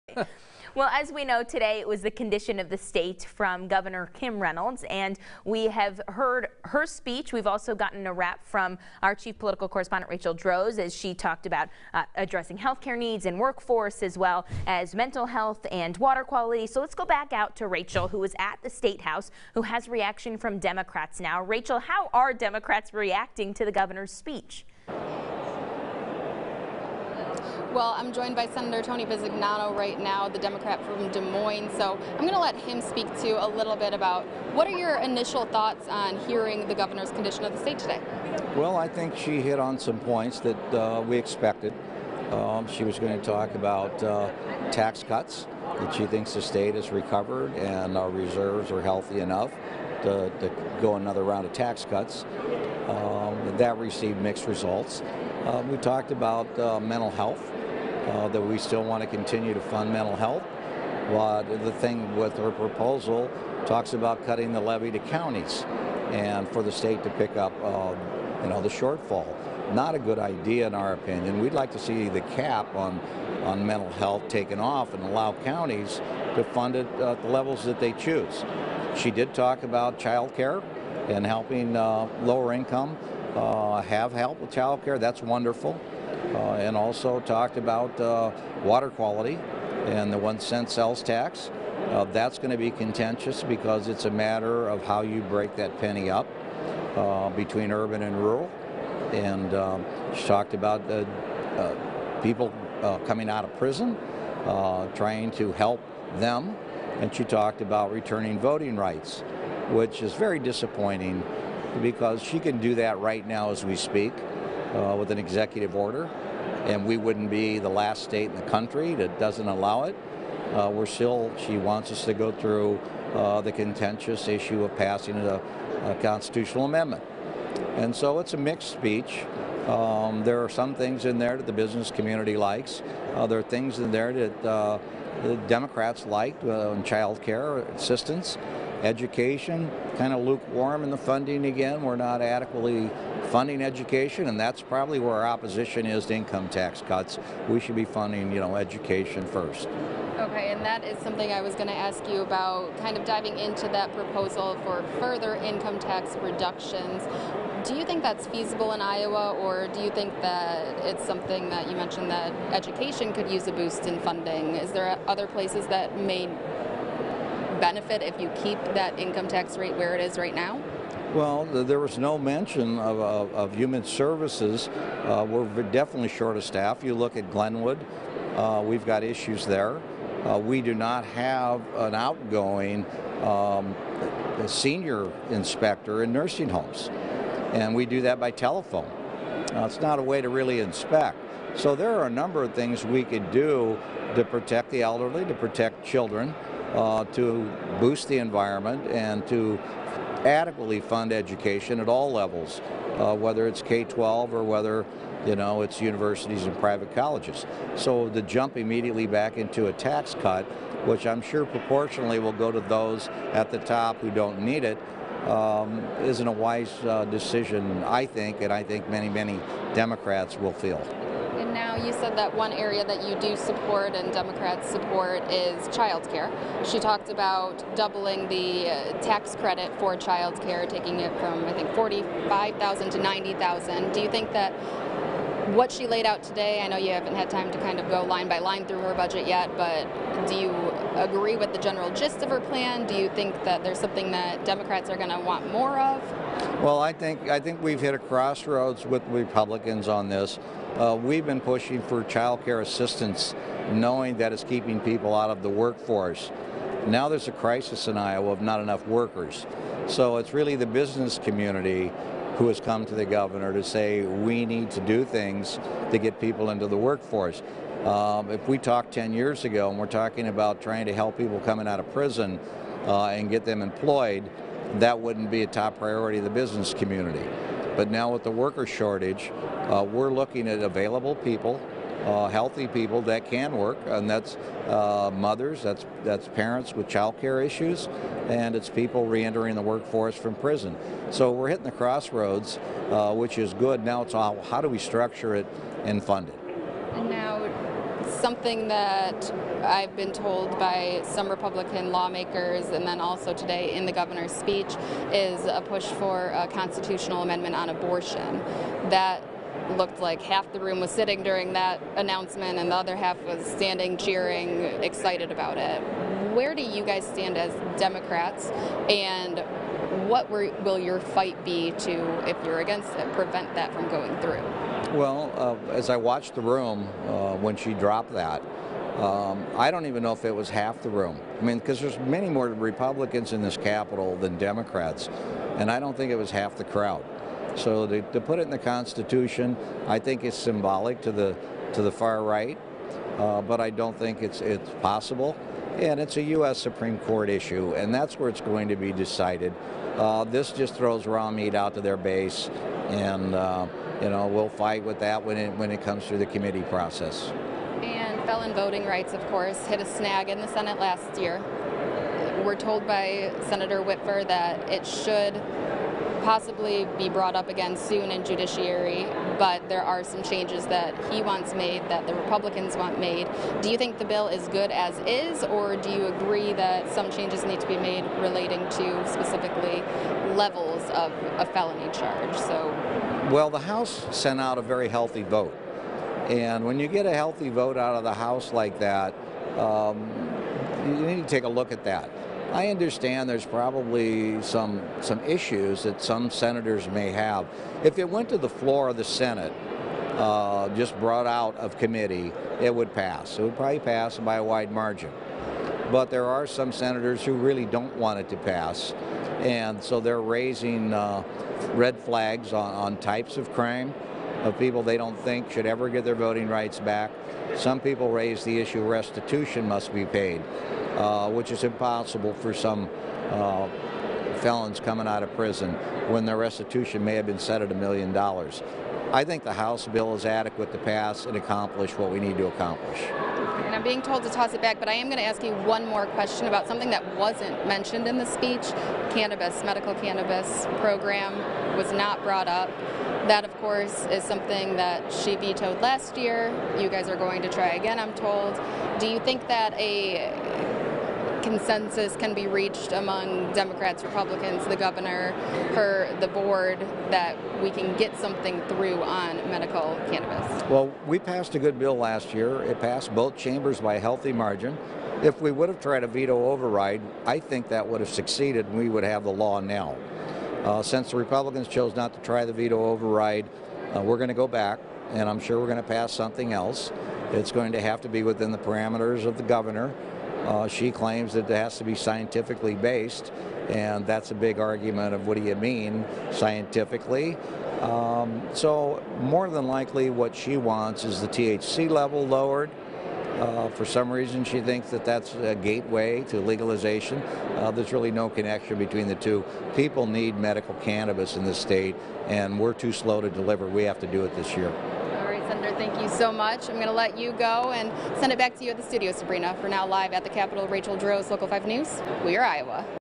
Sen. Tony Bisignano reacts to 2020 Condition of the State
Gov. Reynolds focuses on tax cuts, mental health funding in 2020 Condition of the State